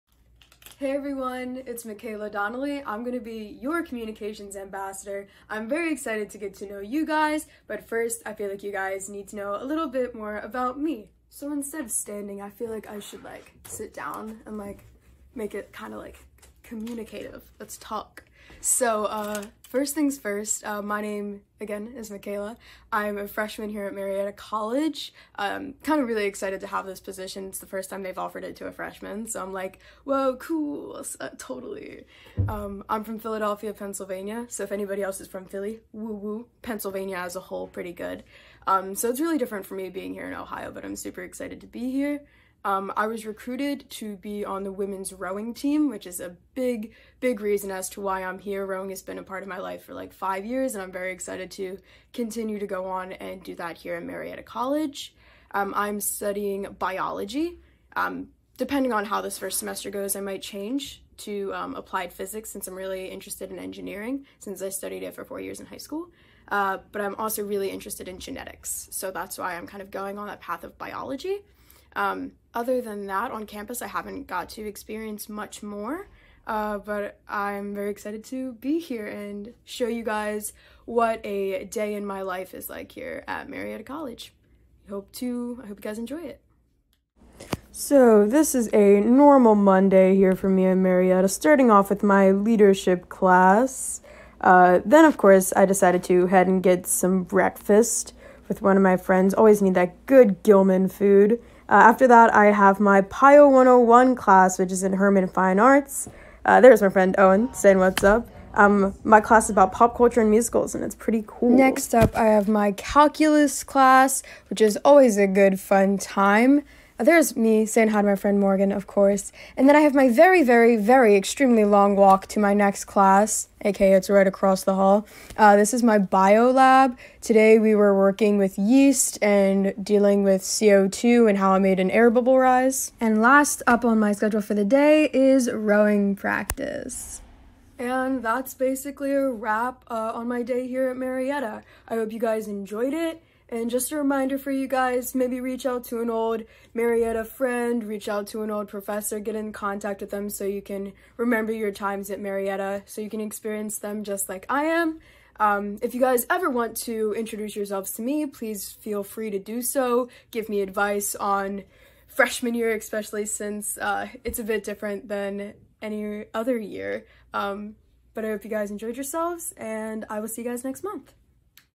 Michaela Donnelly, MC '24 Introduces Herself!
My name is Michaela Donnelly and I will be your student ambassador this year. I am a first-year student here at Marietta College and I’m super excited to grow and be part of such a wonderful community. With having a very different-looking senior year of high school, I am happy to be back to some kind of “normal” for my first year of college. I’m so grateful to be able to have in-person classes, thanks to everything the College is doing to keep us safe. So far the transition has been smooth and I can only imagine all the good things to come!

I’m excited to get to know all of you so please don’t hesitate to reach out and introduce yourselves.